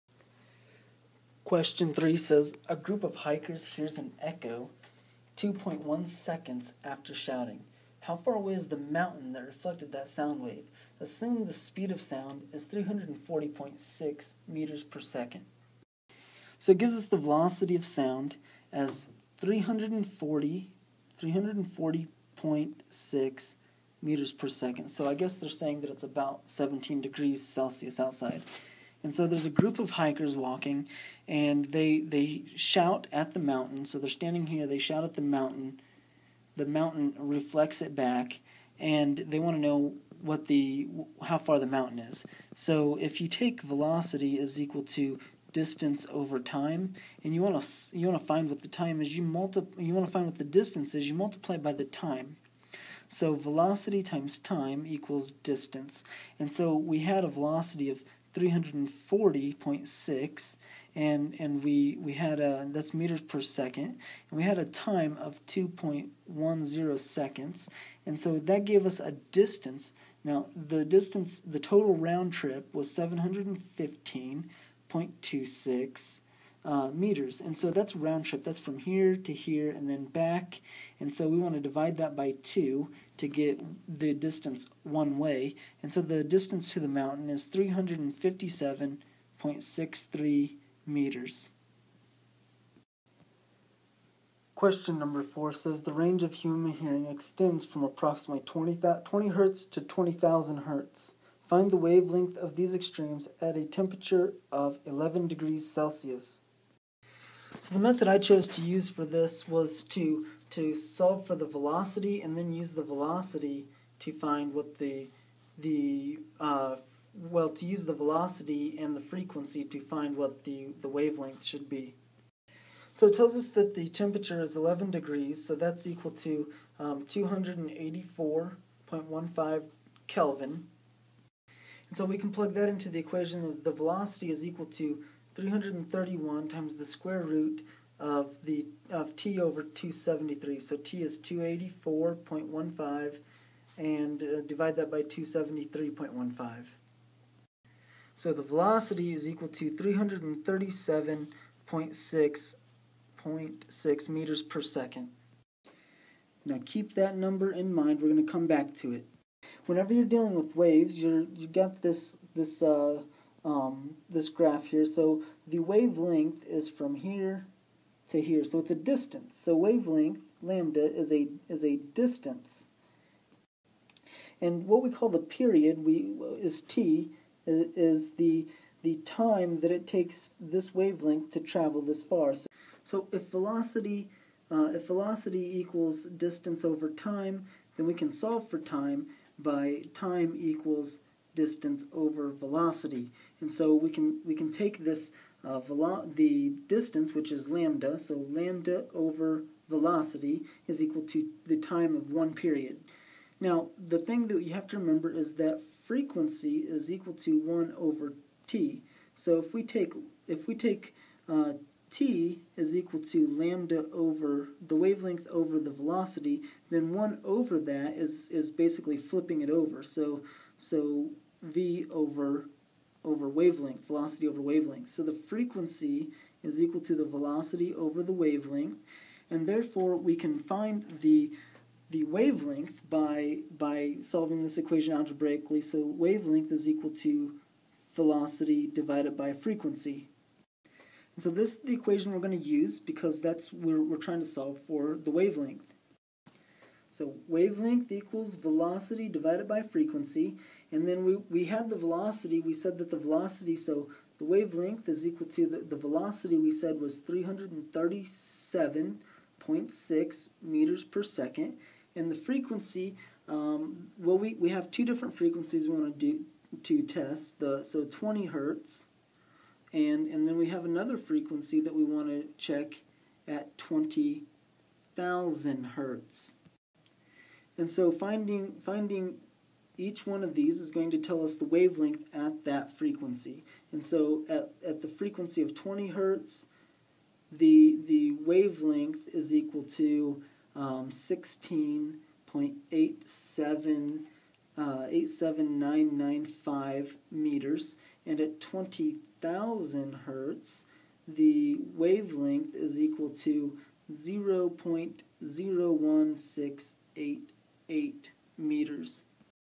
Physics Web Assign Ch 14 #4
For more Chapter 14 Physics homework problems worked out, go

The range of human hearing extends from approximately 20 Hz to 20,000 Hz. Find the wavelengths of these extremes at a temperature of 11°C.
20 Hz        _____ m
20000 Hz  _____ m